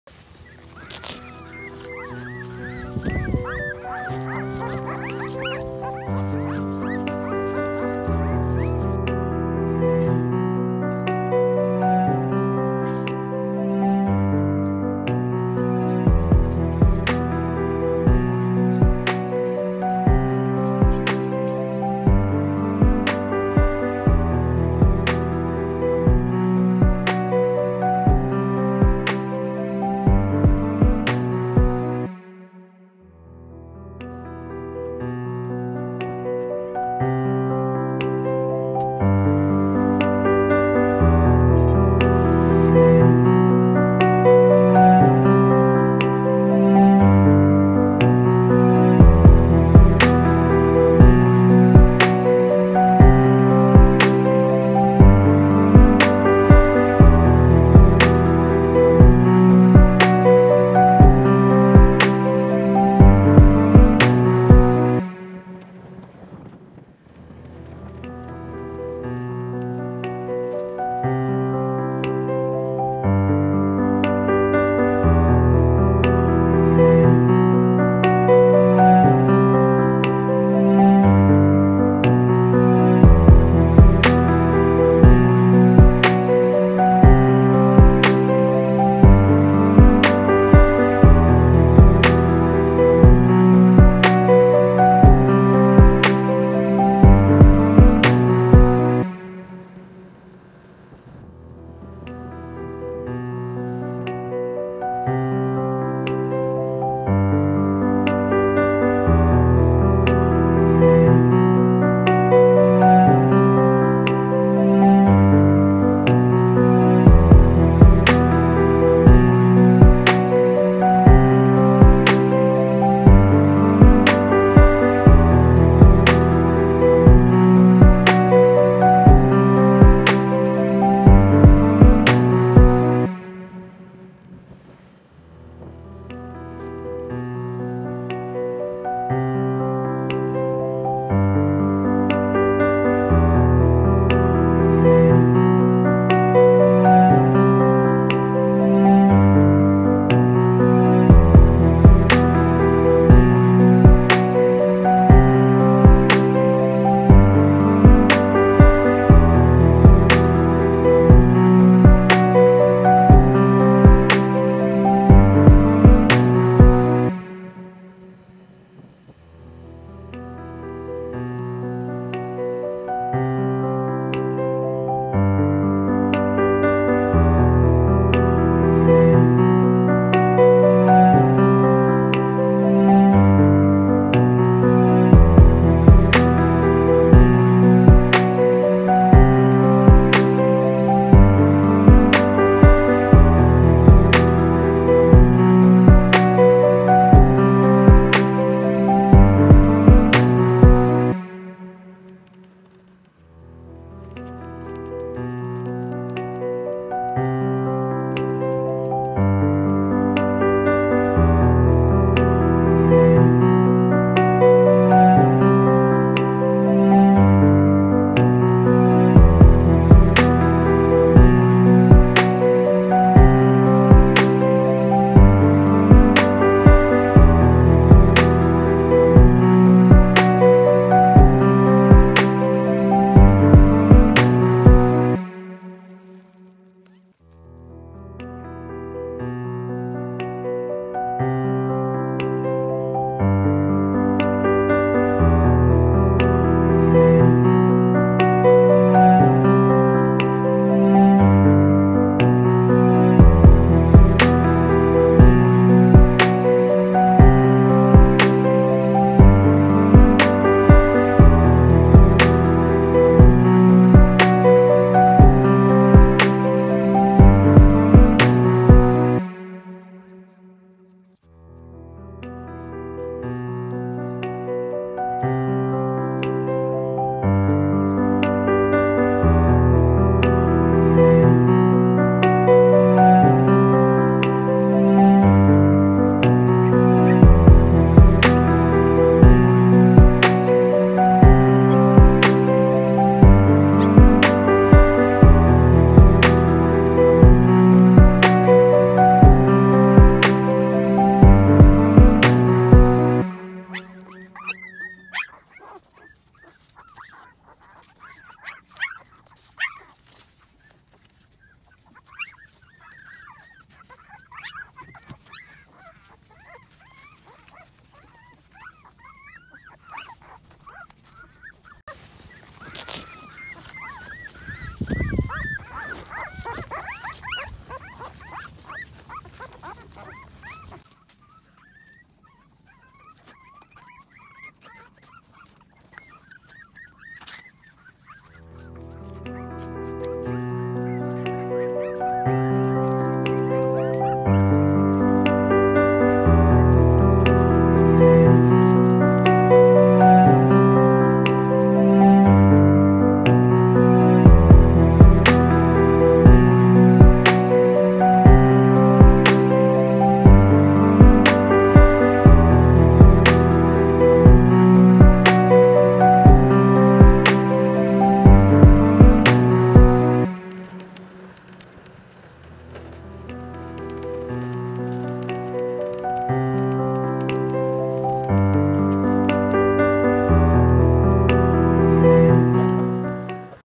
Esp#3 lovely day giving meal for poor poppy’s mom
Hello guy in this video we wanna show how is the poor dog life we just small meal as we can do and meal dog mom so she can get more milk for her babies